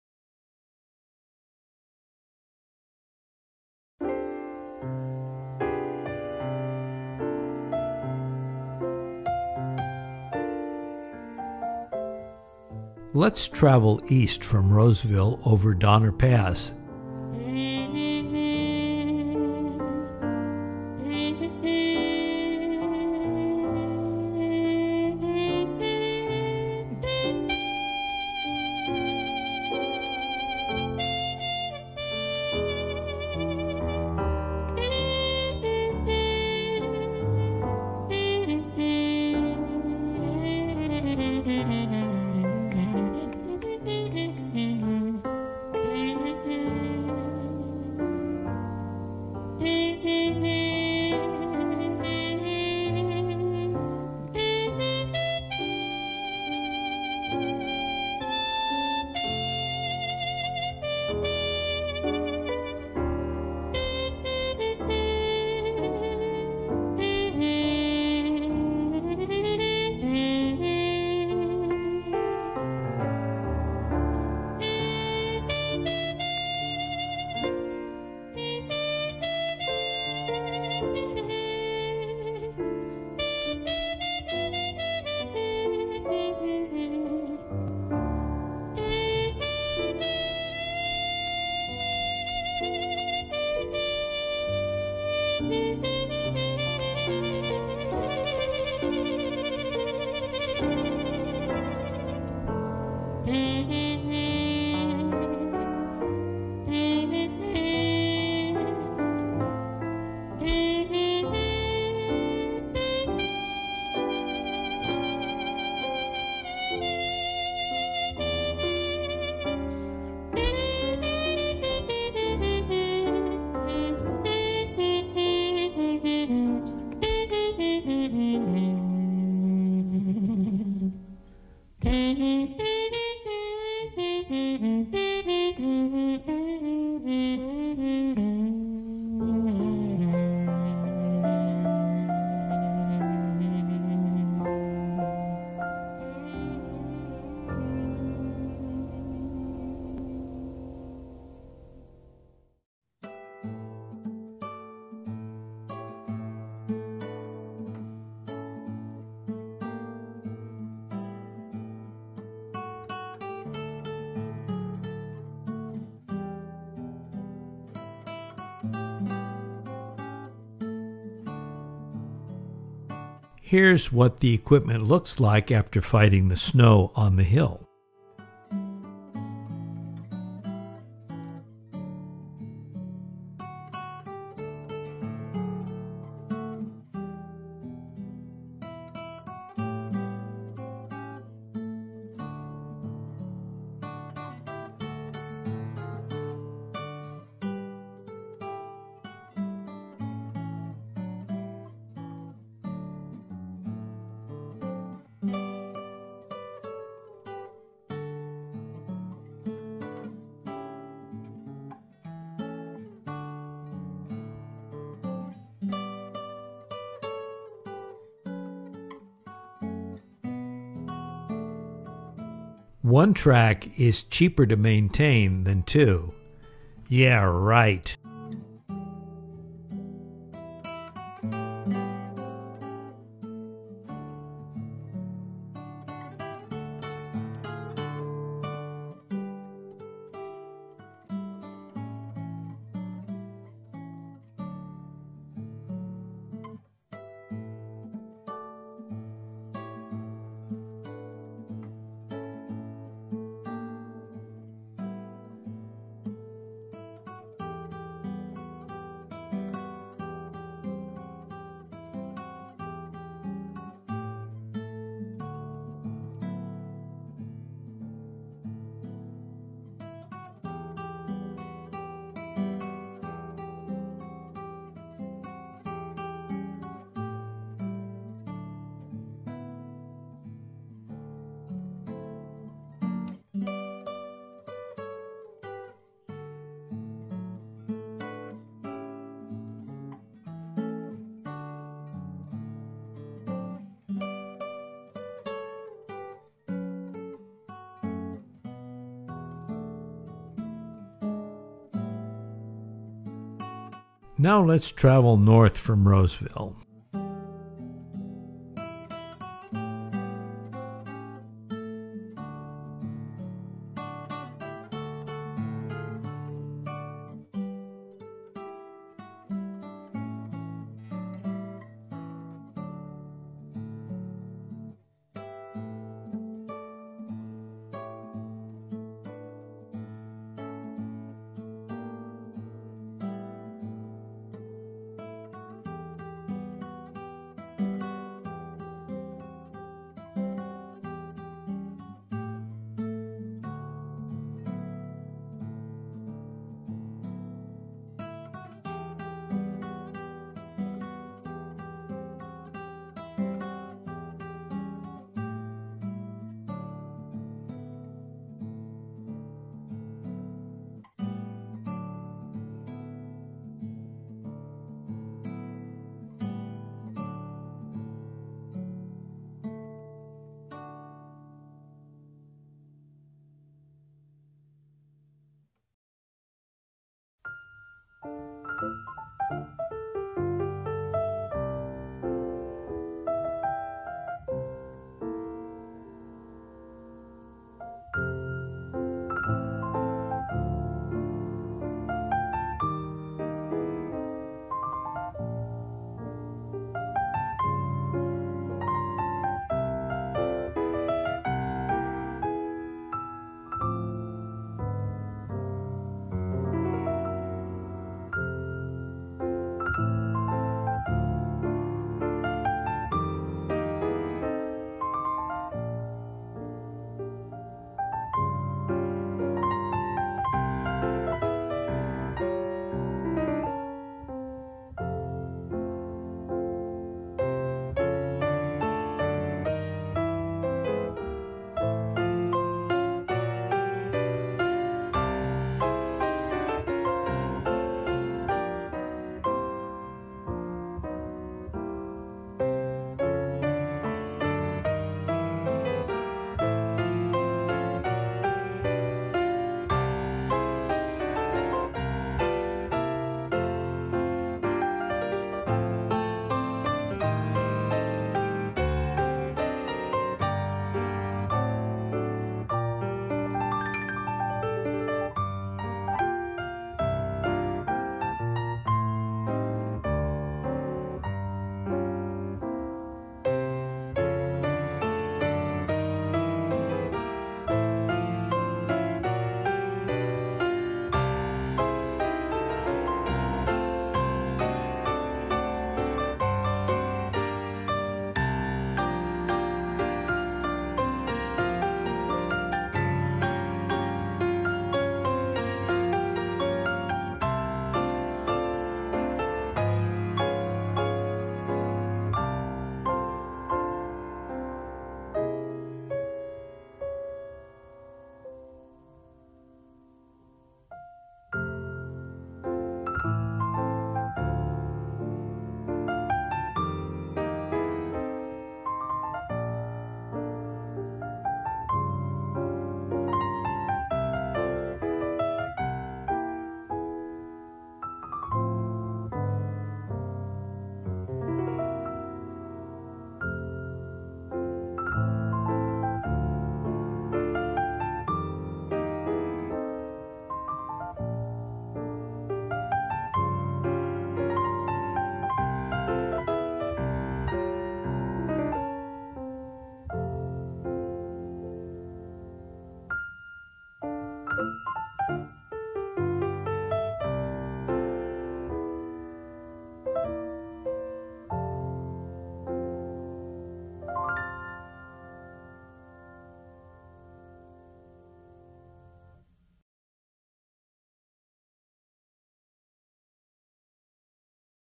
Southern Pacific, East & North from Roseville
East over Donner and north to Mt. Shasta.